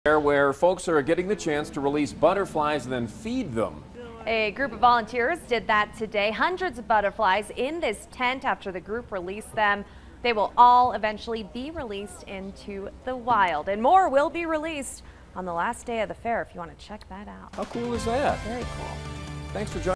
Group releases butterflies at Colorado State Fair
This year at the Colorado State Fair people are getting the chance to release butterflies and feed them. ?